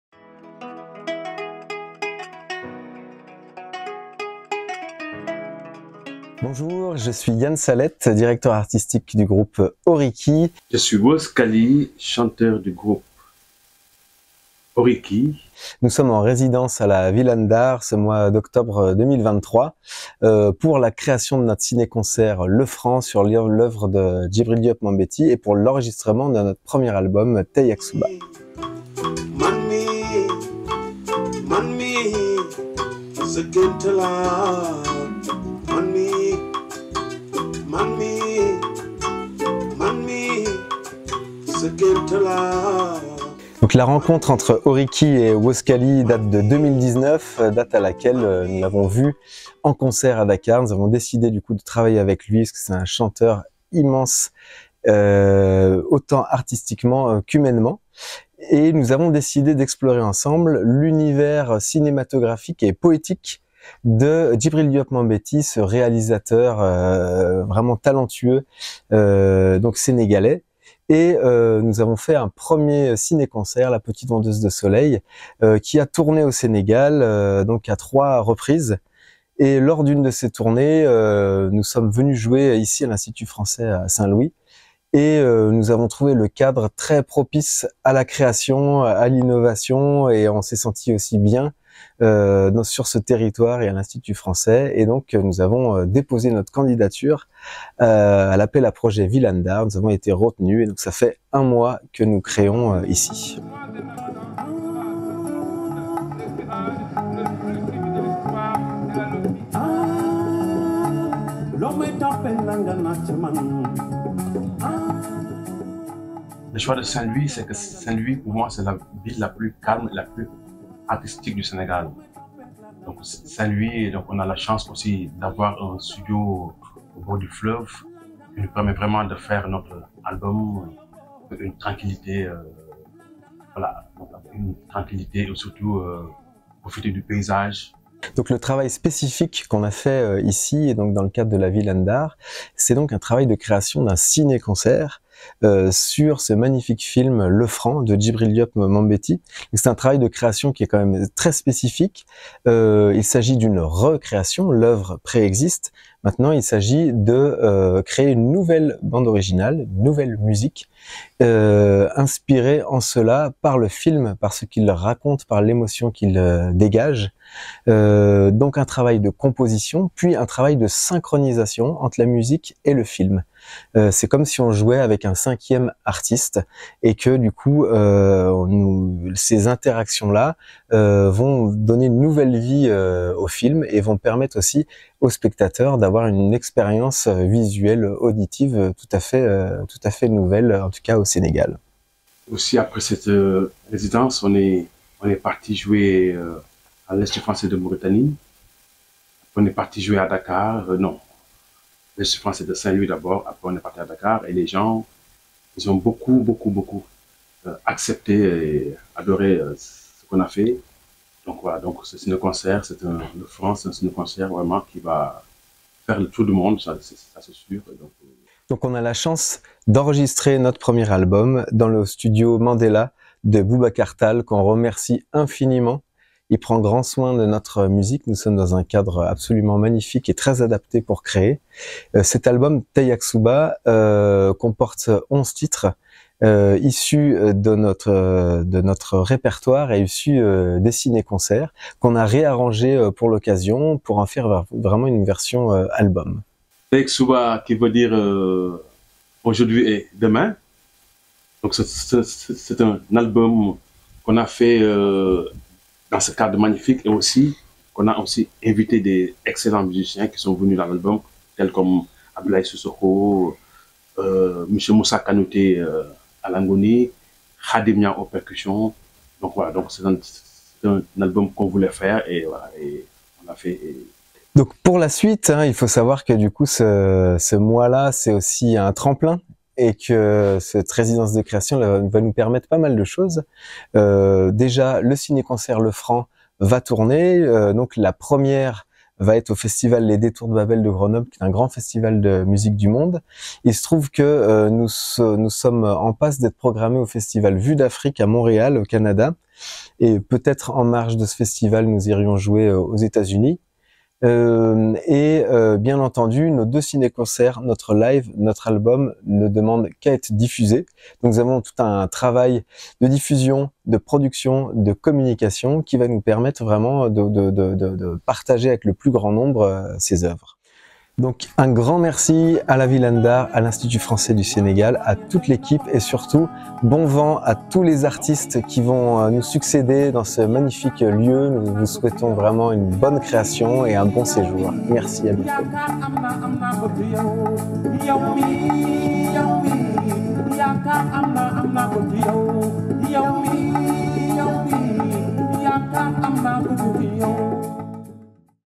ORIKI & WOZ KALY | Retour sur Résidence | Villa Saint-Louis Ndar
En octobre 2023, la Villa Saint-Louis Ndar a accueilli en résidence le trio Oriki et Wolz Kaly. Ils ont mené ensemble un travail de fond sur les musiques orientales et africaines, les chants polyphoniques, des riffs inspirés des musiques traditionnelles…

Les artistes ont développé un nouveau ciné-concert en poursuivant l’adaptation d’une autre oeuvre majeure de Djibril Diop Mambéty : Le Franc. Ce nouveau projet articulera résidences de création artistique, représentations publiques, médiations culturelles, enregistrement phonographique et coopération internationale sur les saisons 2023 et 2024.